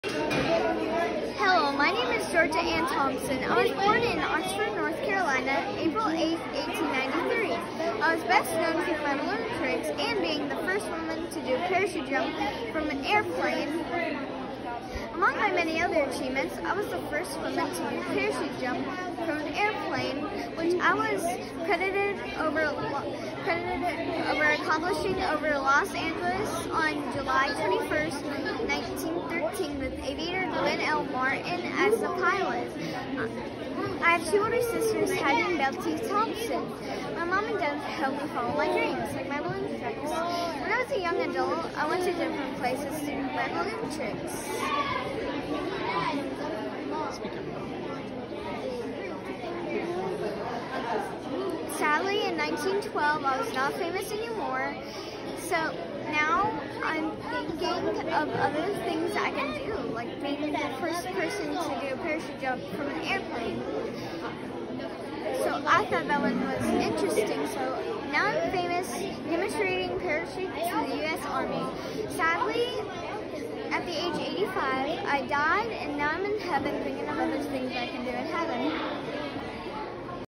Loralei wax museum Georgia Ann Thompson 4/14/2022
Loralei’s school, Mills River Elementary does a wax museum project every year and 4th graders are assigned a person from North Carolina to research and dress up as. They then have one day where they will hold up their hands for people to hit their button to hear the facts about their person.